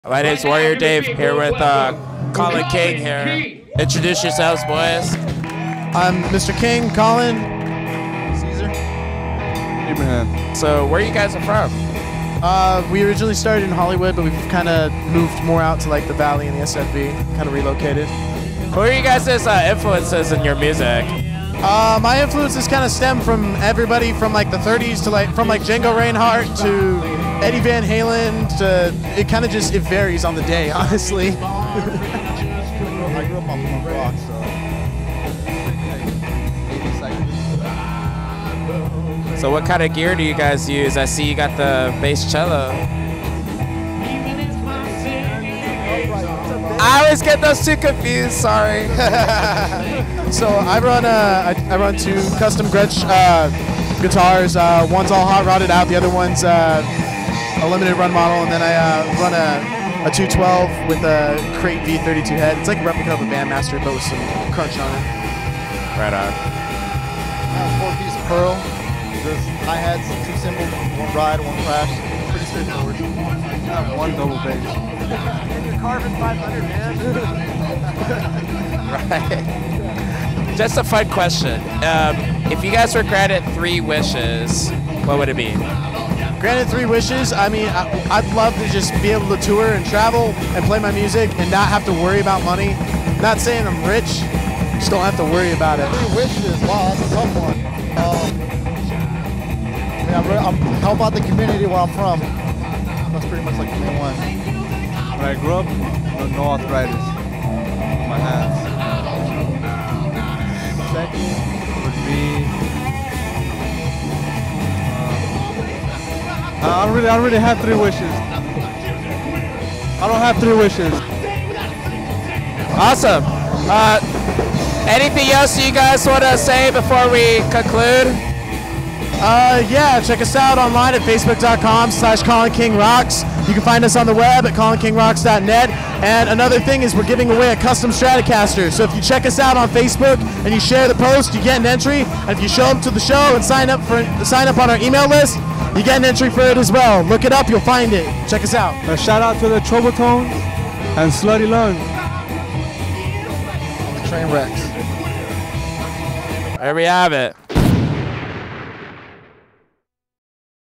Interview with Collin King - Killarnery's - Huntington Beach, CA
Follow Collin King as they advance on to the second round of Battle for Vans Warped Tour 2016, presented by Acropolis Records and Skinnie Magazine.